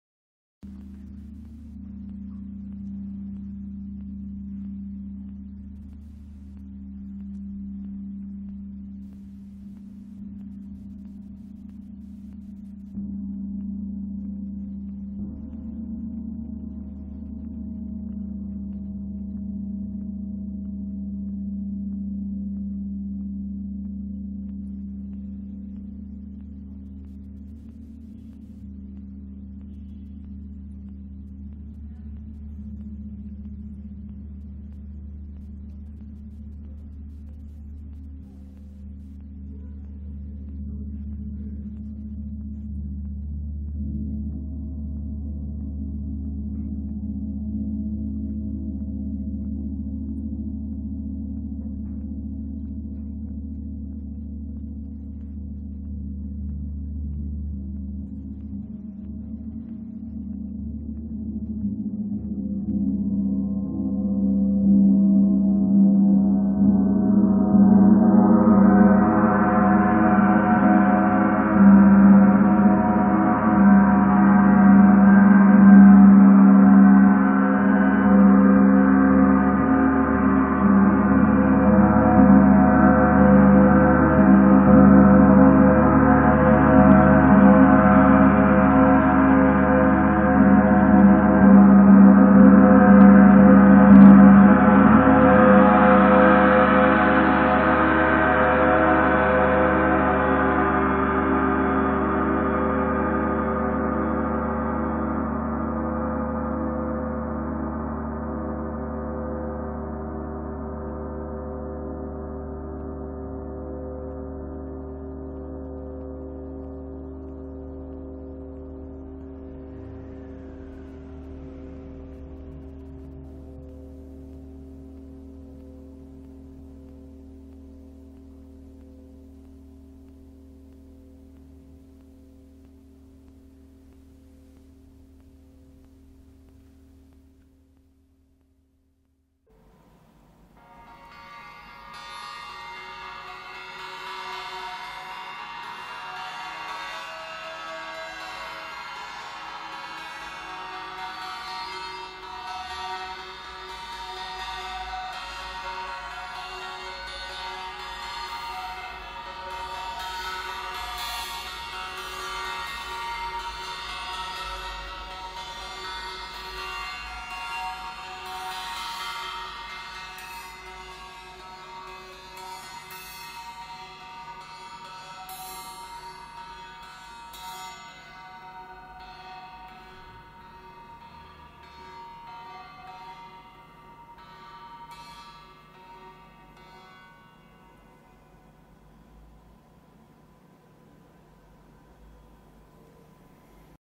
First Touch on Saturn Paiste Gong - Art of GongLove Training 2013-2014
[EL] Η πρώτη φορά που ο Νίκος Κυριαζής συναντήθηκε και άγγιξε το πλανητικό γκονγκ του Κρόνου της Paiste.

Aπό το αρχείο της πρώτης ολοκληρωμένης εκπαίδευσης στην Τέχνη του Γκονγκ στην Ελλάδα από το Εργαστήρι της Τέχνης των Γκονγκ GongLove ArtLab - Δεκέμβρης 2013